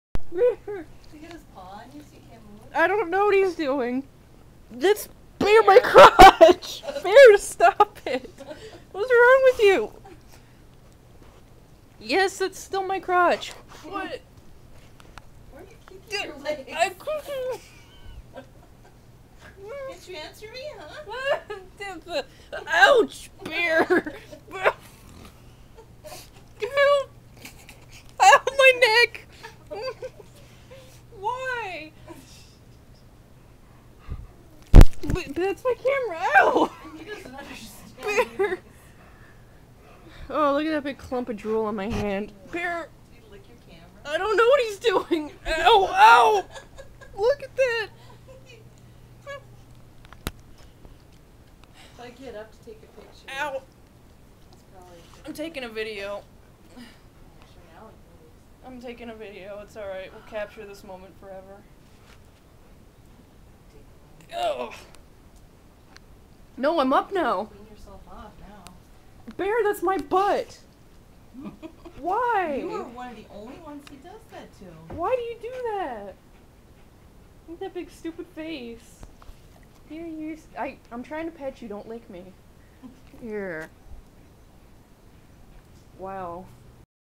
Bear attack
If he sees you're at his level, this is what he does.